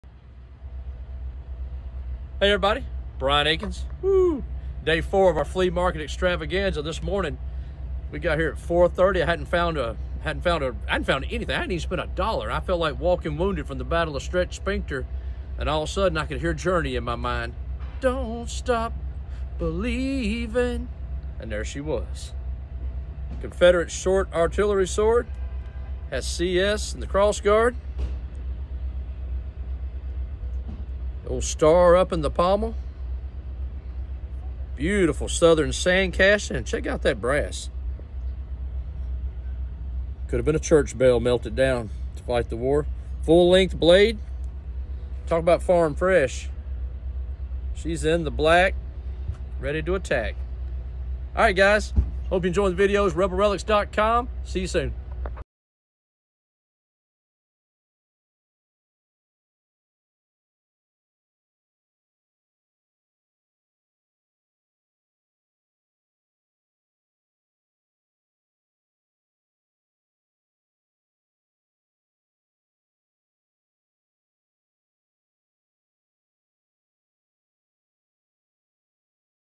Confederate Artillery Sword
Brimfield Flea Market Pick …. Gorgeous untouched Confederate Short Artillery Sword. Love the patina and attic condition. brianakins civilwar civilwarrelics confederate brimfield #2023